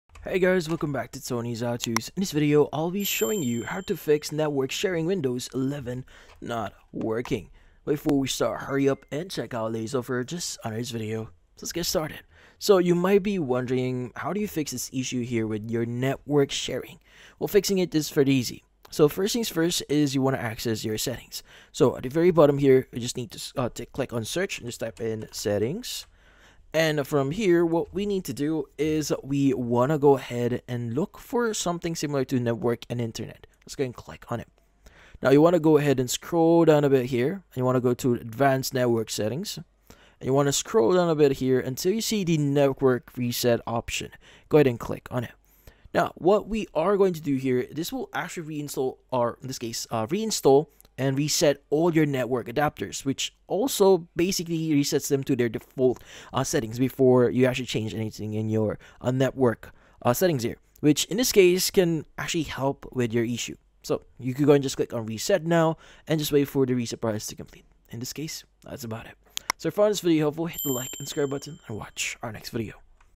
How to Fix Network Sharing Windows 11 Not Working (2026)
In this video, I show you How to Fix Network Sharing Windows 11 Not Working in 2026. The quick and easy tutorial Fixed network sharing not showing all computers windows 11. Do you want to know step by step how to solve cannot share internet via hotspot in 2026? Yes? Great!